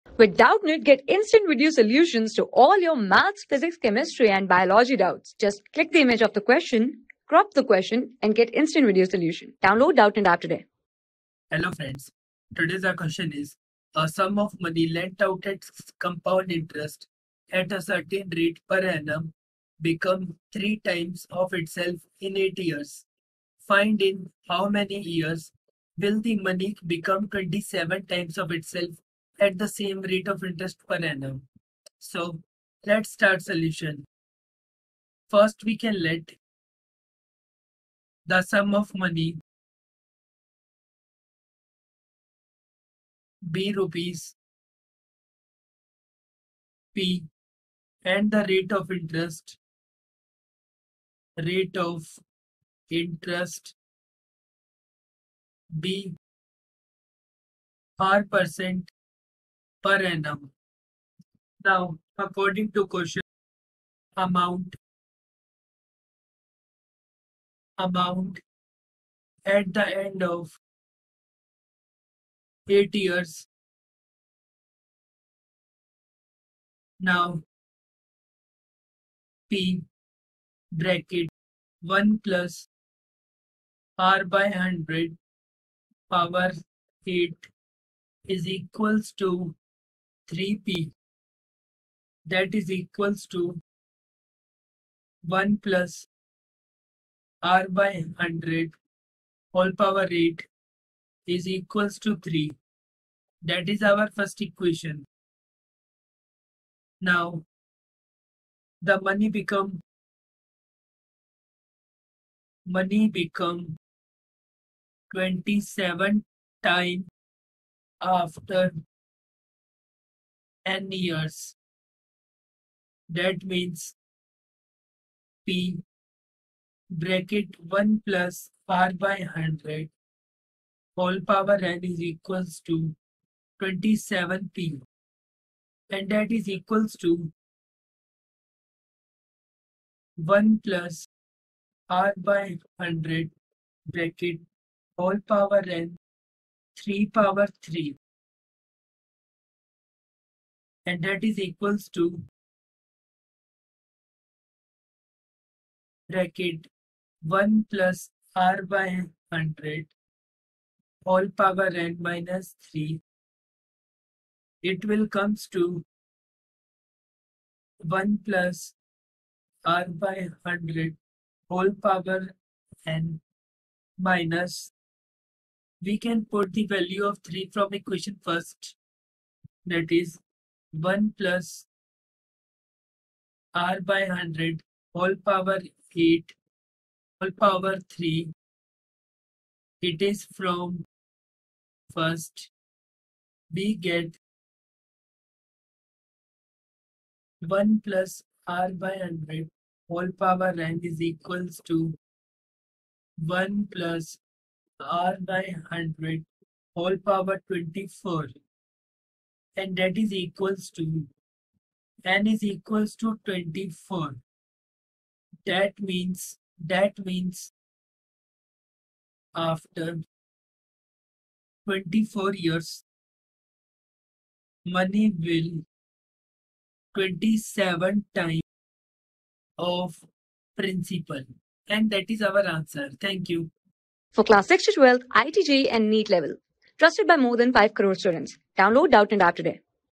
A sum of money lent out at C.I. at a certain rate per annum becomes three times of itself in 8 y...
A sum of money lent out at C.I. at a certain rate per annum becomes three times of itself in 8 years. Find in how many years will the money become twenty-seven times of itself at the same rate of interest p.a.
Class: 9
Subject: MATHS
Chapter: COMPOUND INTEREST (USING FORMULA)
Board:ICSE

👉 Have Any Query? Ask Us.